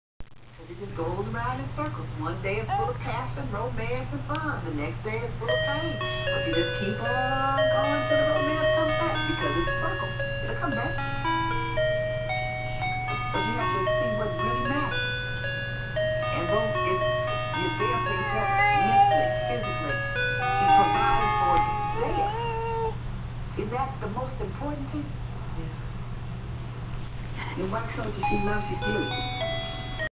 Izaak w/ music pad
just short clips of Izaak mainly for the grandparents, T.T.'s brother, and sisters, etc.. to see